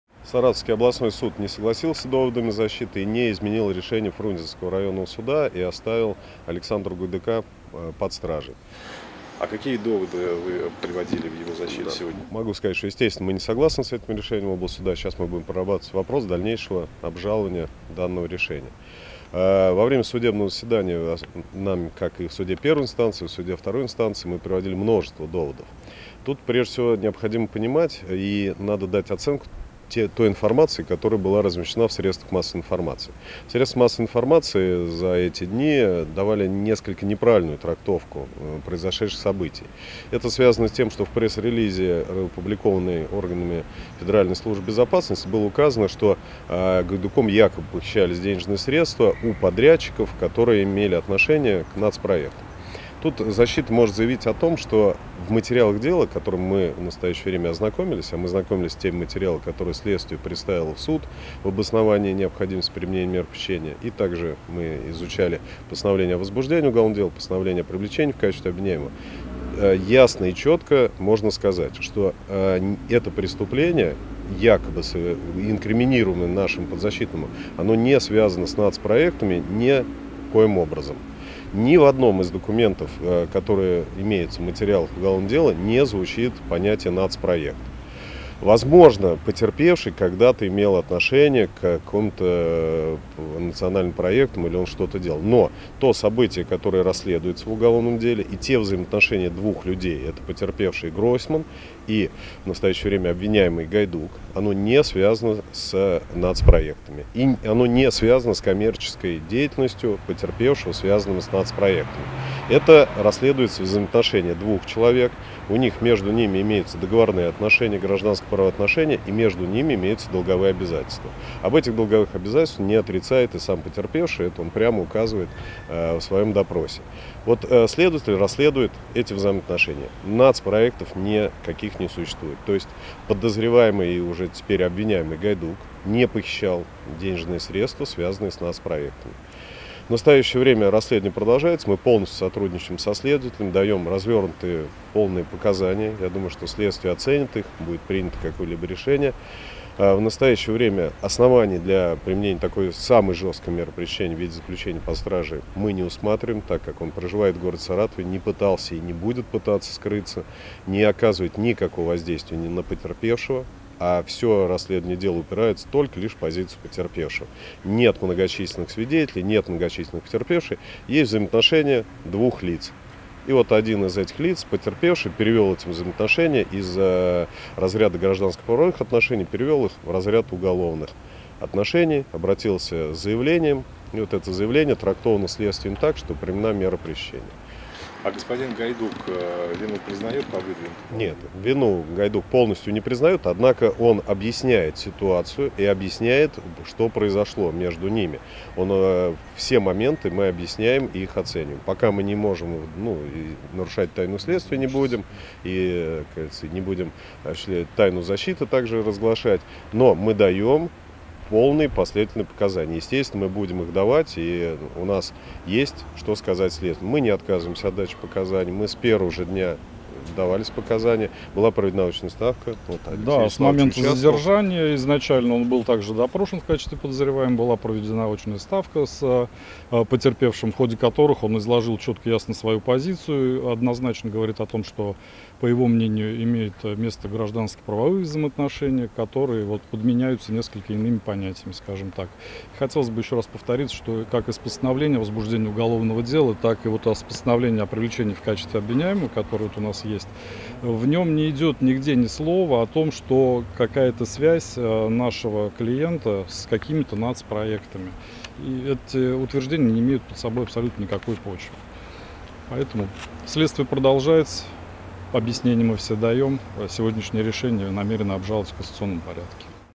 Заявление адвокатов Александра Гайдука
Адвокаты Александра Гайдука заявили о непричастности своего подзащитного к хищению денег, выделенных на реализацию нацпроекта "Жилье и городская среда".

Об этом защитники рассказали в комментарии ИА "Взгляд-инфо".